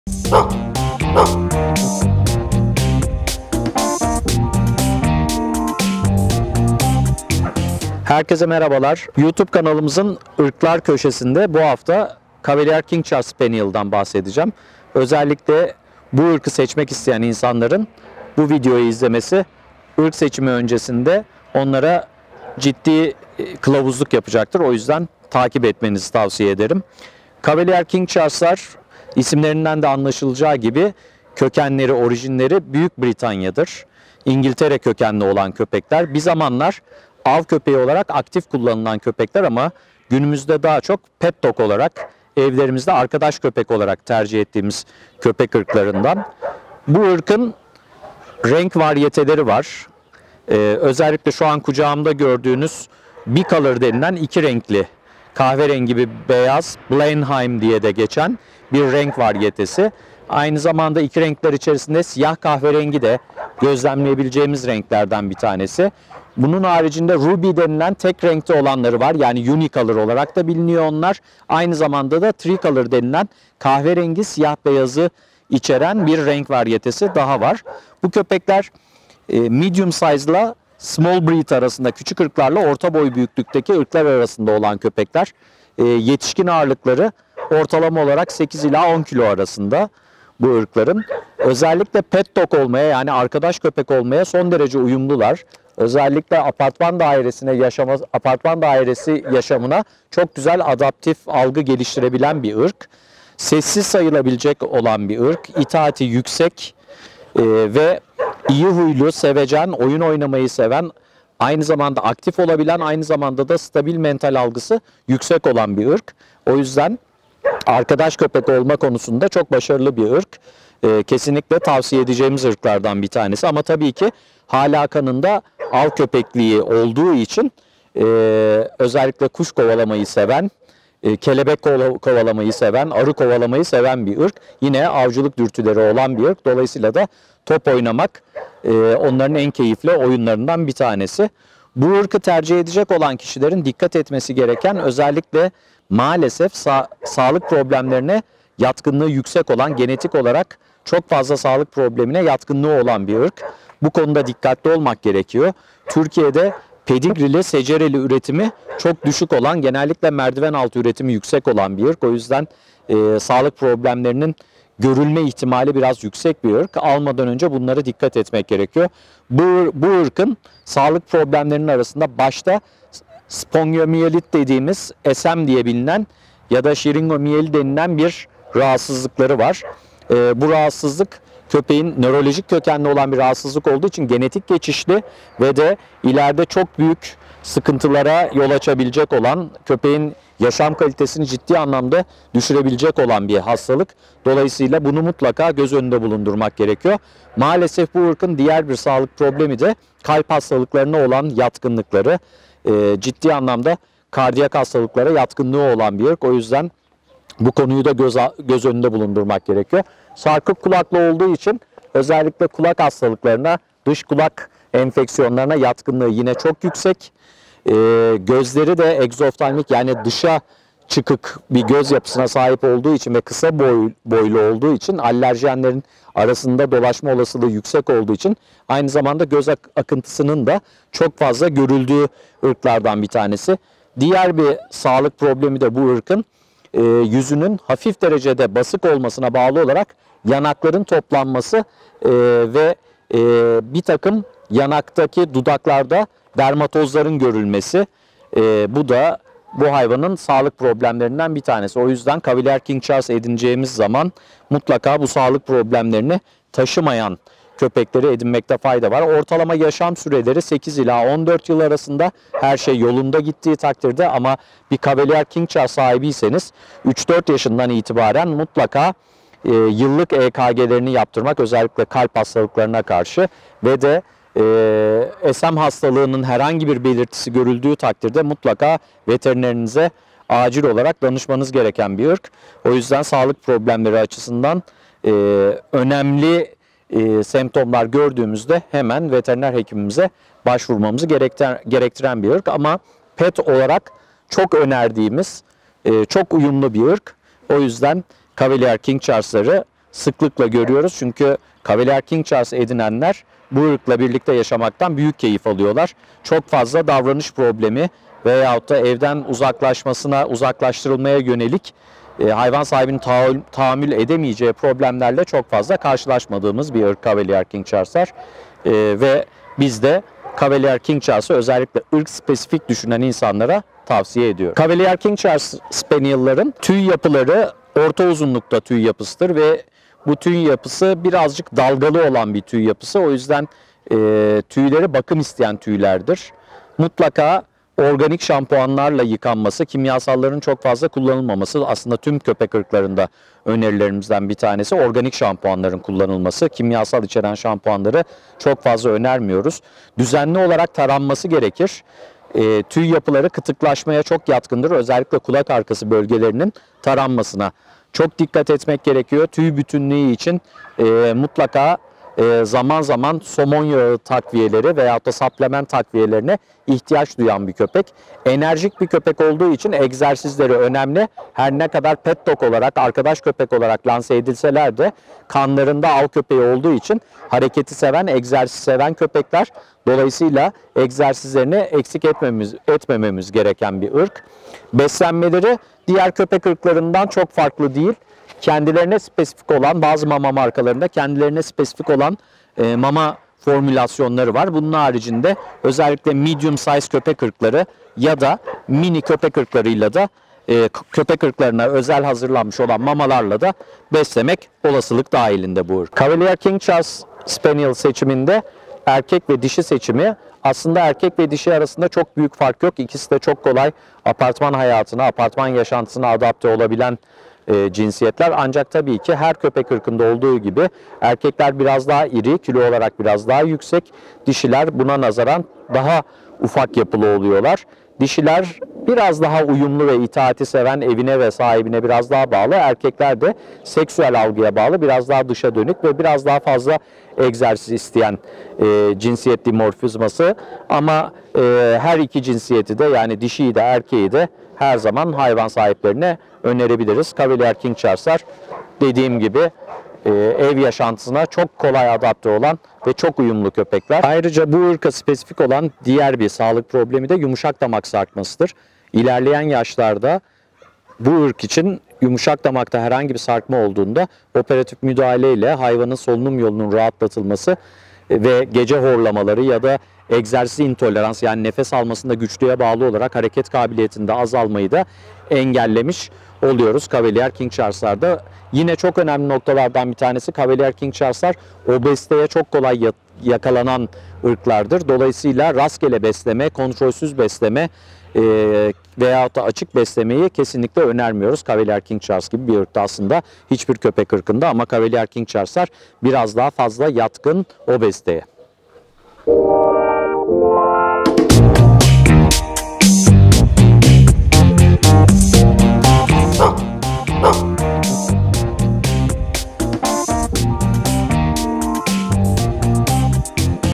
Köpek Irkları - Cavalier King Charles Spaniel
Her hafta bir köpek ırkına yer verceğimiz listemizde, bu hafta sizlere Cavalier King Charles Spaniel' ı anlatıyoruz. Çevrenizdekileri bilinçlendirmek adına paylaşınız lütfen. Sizde merak ettiklerinizi yorum kısmına yazabilir, sorularınızı bizlere ulaştırabilirisiniz.